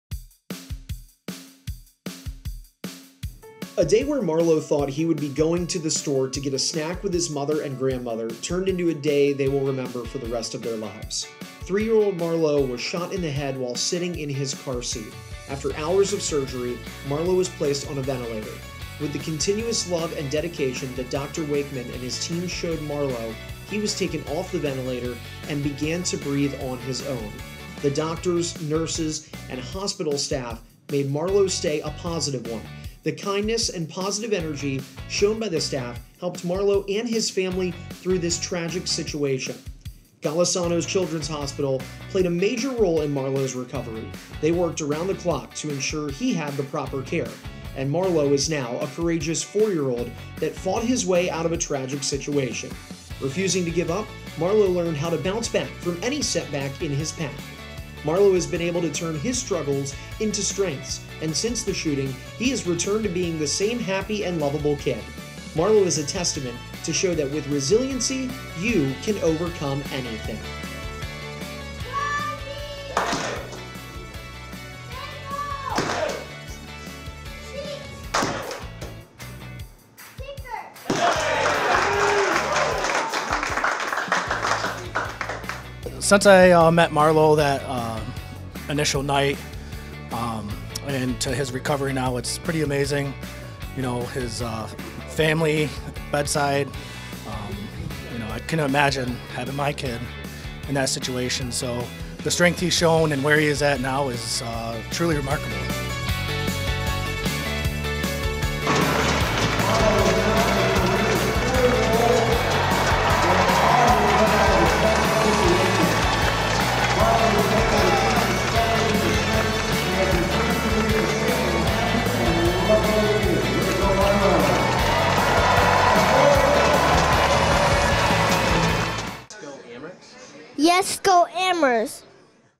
2023 Rochester Americans Honorary Captain: Marlo Joseph
Last year, Marlo Joseph survived a harrowing gunshot wound to the head while out with his family.  Thanks to the work of the GCH trauma and surgery teams, he is now doing great, and he got to join the Rochester Amerks on the ice as honorary captain!